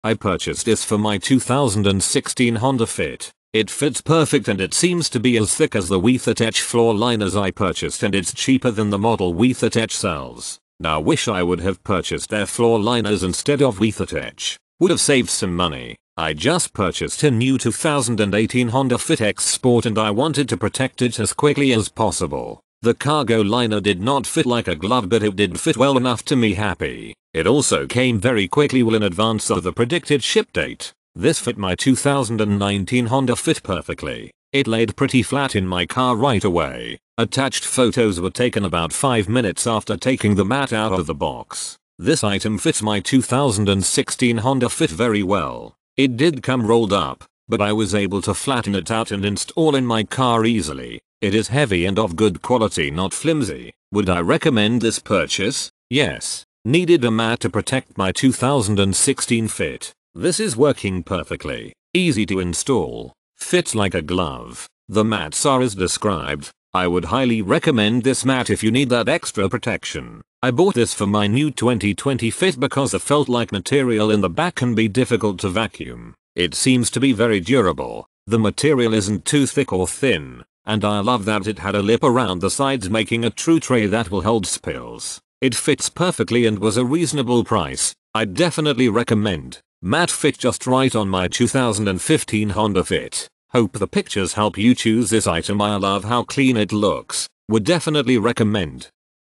MAXLINER All Weather Custom Fit Cargo Liner Behind The 2nd Row Black Compatible review
MAXLINER All Weather Custom Fit Cargo Liner Behind The 2nd Row Black Compatible with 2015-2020 Honda Fit

Main Features:
- Compatible With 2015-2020 Honda Fit
- ALL WEATHER PROTECTION - All-weather, stain-resistant protection that gives your car, van, truck, or SUV interior a first class look while protecting its resale value.
- CUSTOM FIT - The raised lip and custom design constructed using innovative vehicle scanning technology ensures the perfect fit that protects your vehicle's carpet floor from the weather, spills and any messes.
- EASY TO INSTALL & CLEAN - Textured, skid resistant surface made from 100% recycled materials offers 100% odorless protection that is easy to clean by hosing off or using soap and water.
- LIFETIME WARRANTY - Strong, durable, easy to install, and backed by a limited Lifetime Warranty.